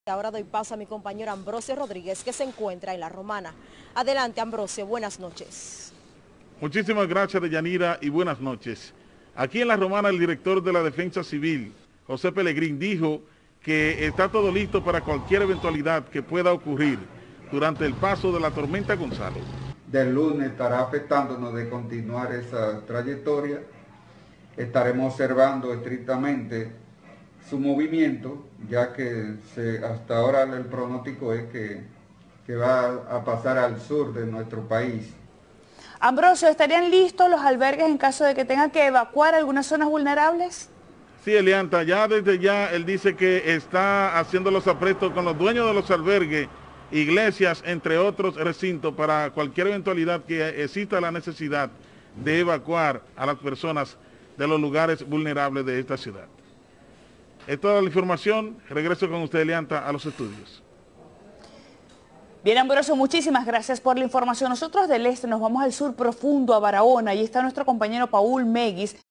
Según la defensa Civil de La Romana todo está listo ante el posible paso de la tormenta Gonzalo.
El Director de La Defensa civil de La Romana dijo que: tiene todo listo para cualquier eventualidad que en lo adelante pueda ocurrir, durante el paso de la tormenta Gonzalo.